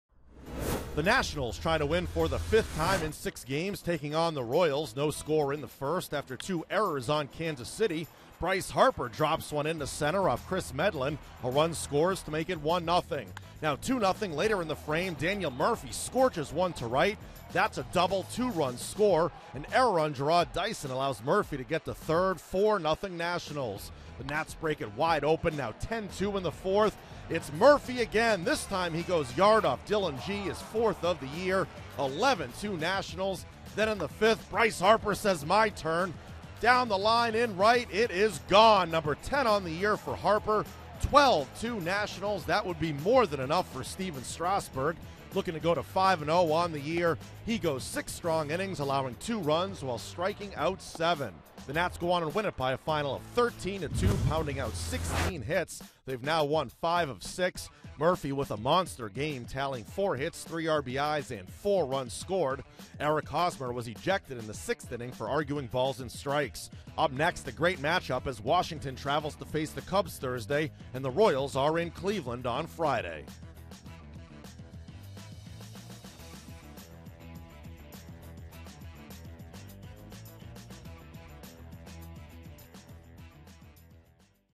5/4/16: Murphy leads Nats over Royals with four hits
Daily Recap: Daniel Murphy and Clint Robinson each collected three RBIs in front of six strong innings from Stephen Strasburg